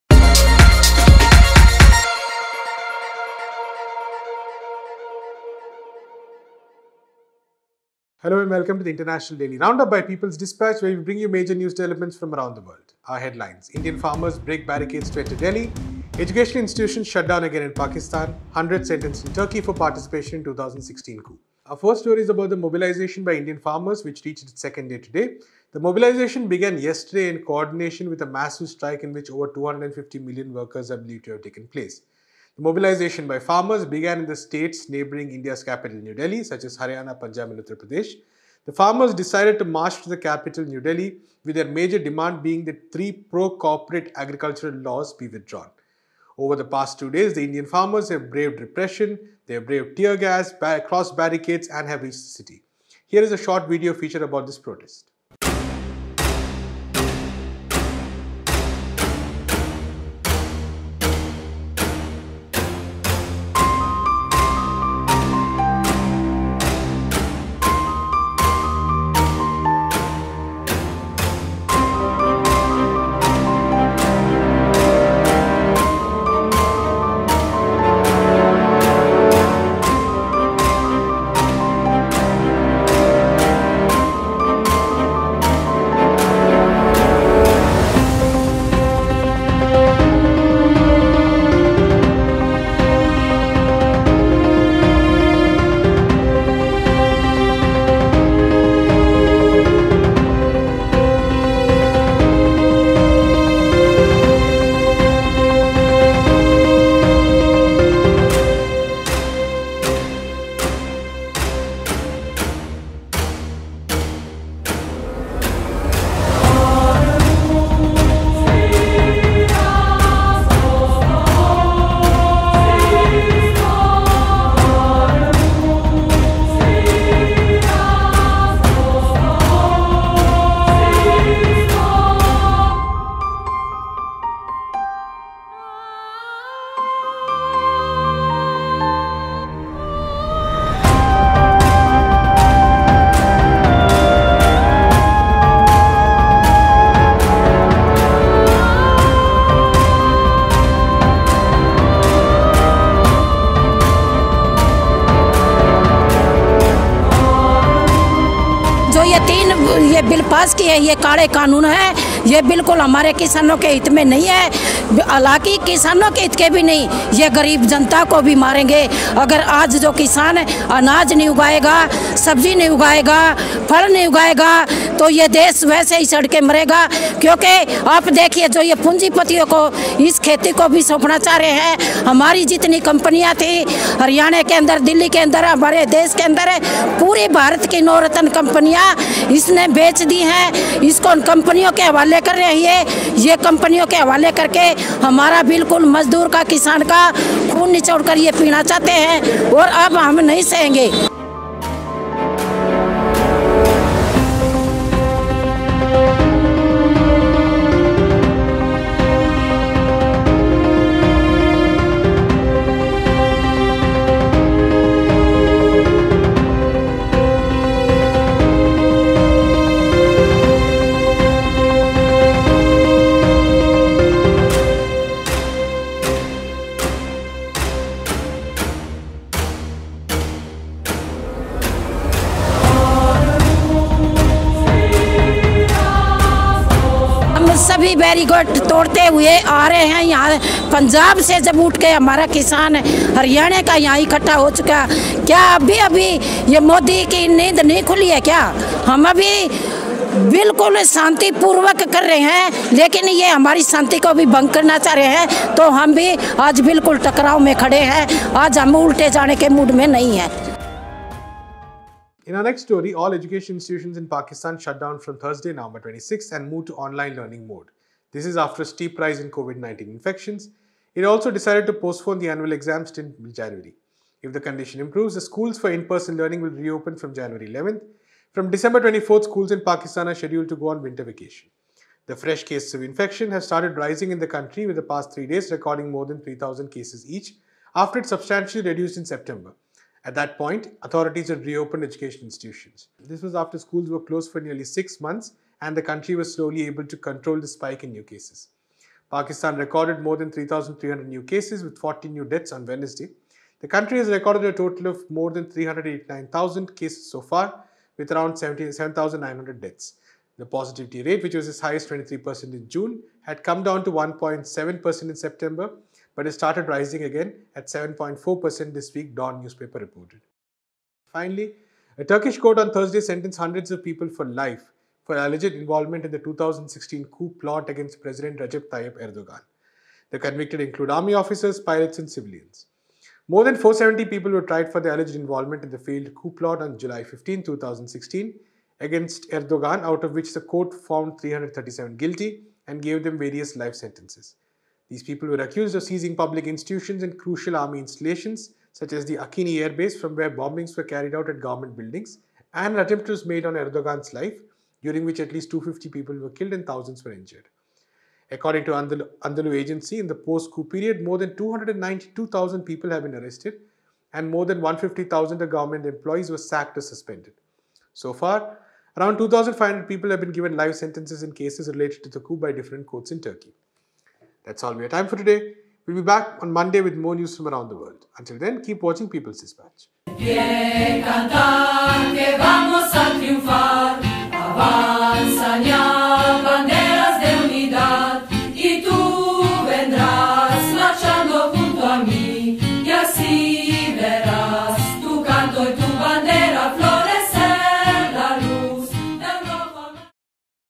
Indian farmers break barriers to enter Delhi
In this episode, we look at the ongoing farmers’ protest in India, the shutting down of educational institutions in Pakistan and the sentencing of hundreds in Turkey for participating in a coup.